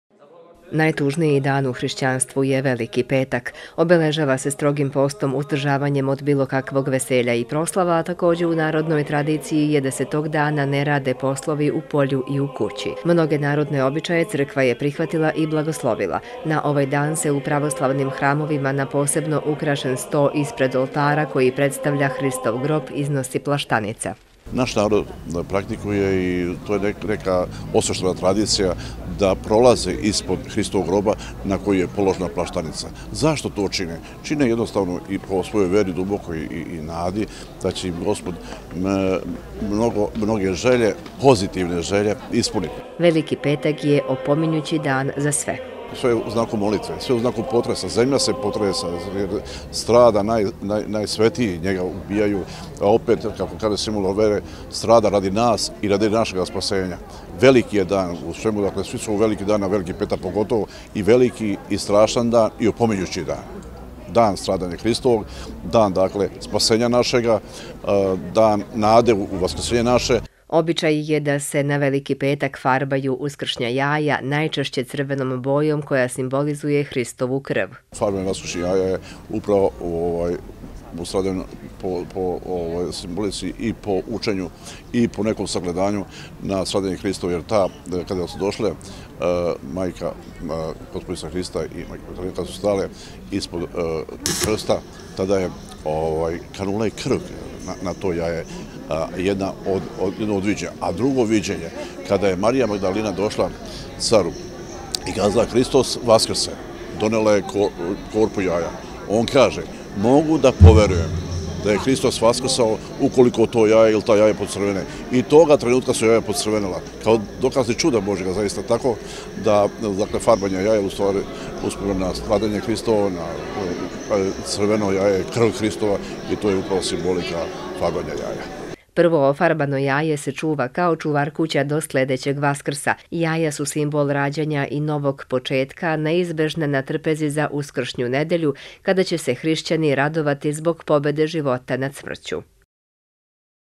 Veliki petak
Hrišćani širom sveta danas obeležavaju Veliki petak, u znak sećanja na dan kada je Isus Hristos raspet na krstu na Golgoti. Za ovaj dan vezani su i  mnogi narodni običaji, koje je crkva blagoslovila a jedan od tih jeste i farbanje uskršnjih jaja.